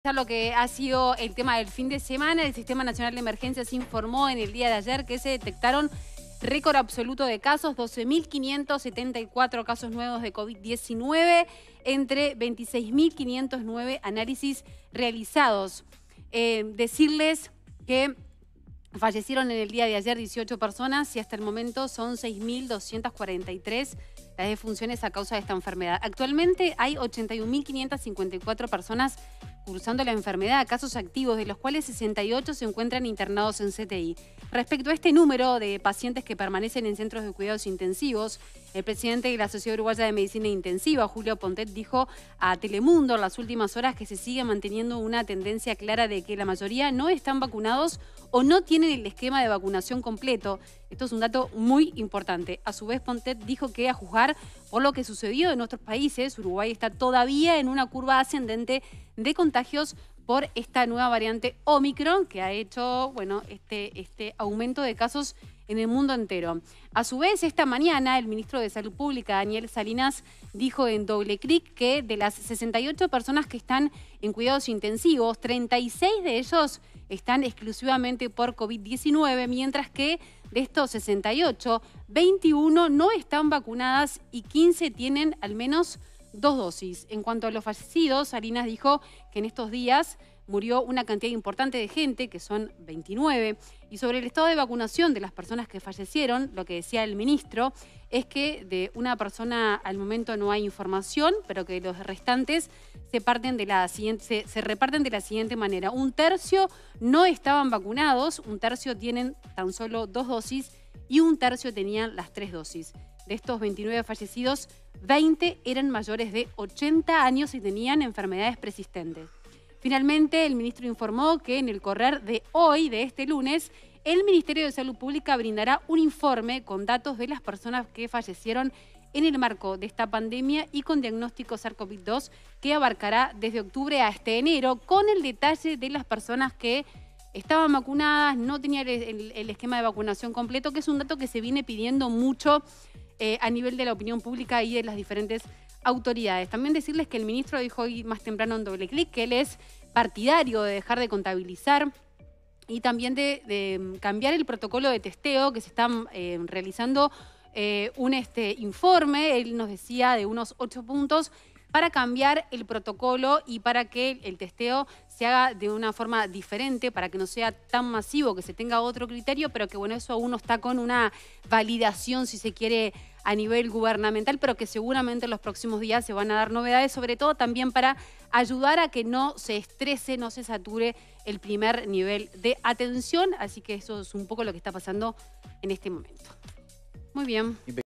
Récord absoluto: 12.574 casos de Covid-19
En la noticia del fin de semana, con Lucía Brocal repasamos el récord de casos positivos que hubo el domingo 16 de enero. Los 12.574 casos se detectaron en 26.509 análisis.